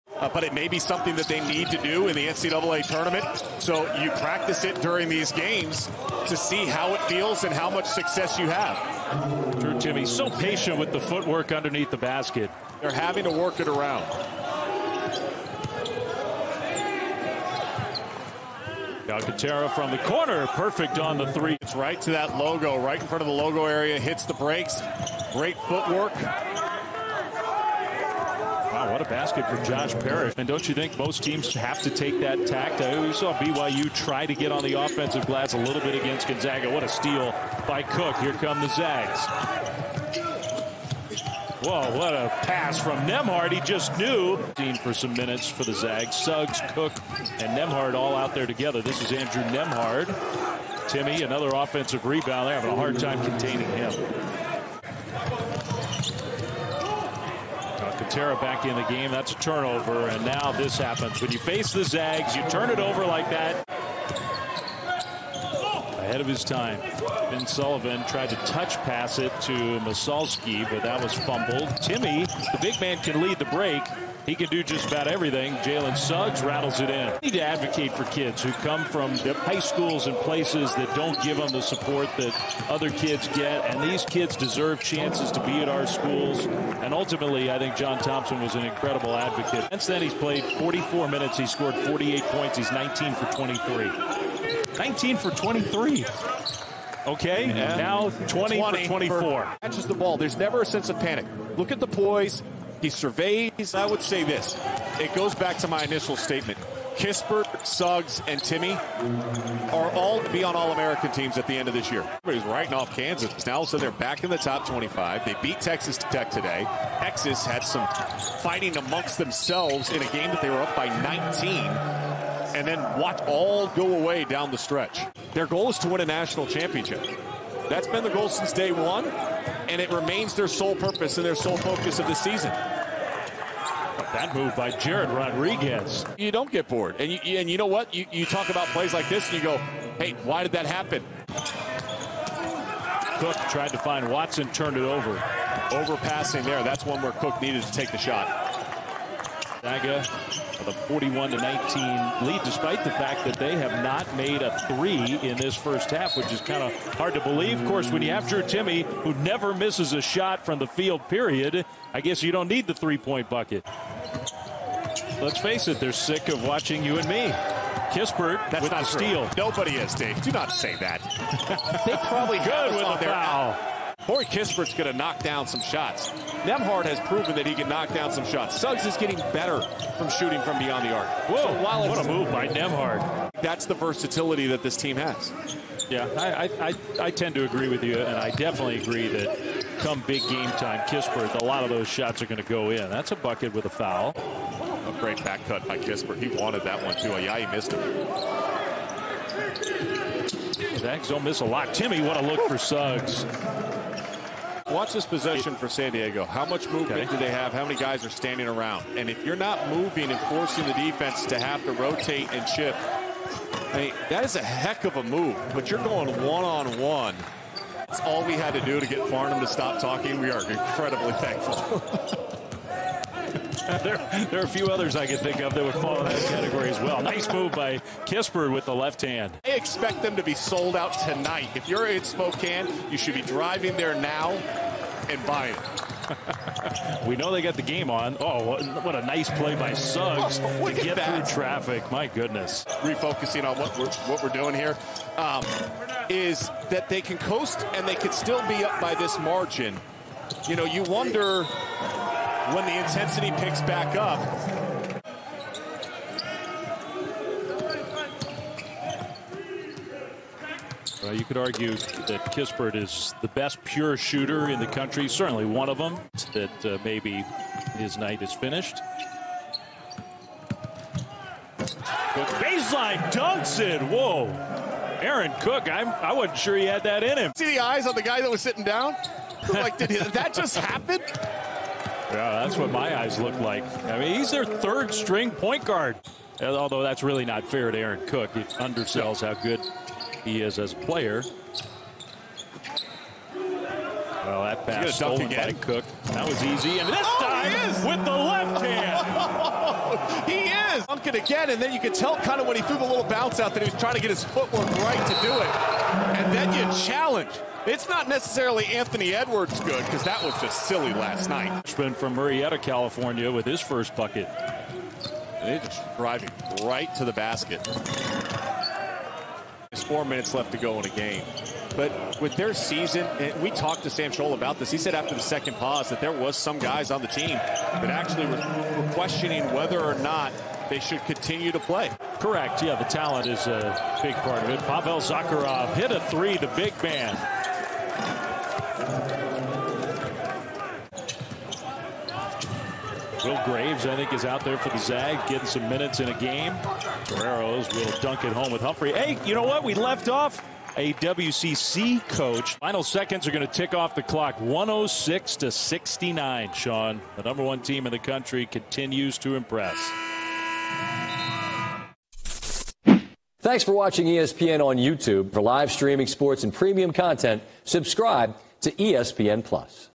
No. 1 Gonzaga crushes San Diego, now 22-0 [FULL GAME HIGHLIGHTS] | ESPN College Basketball
Check out highlights from No. 1 Gonzaga, as Drew Timme leads four Bulldog starters who score in double digits in a 106-69 win against the San Diego Toreros and improve to 22-0 in the 2020-21 college basketball season.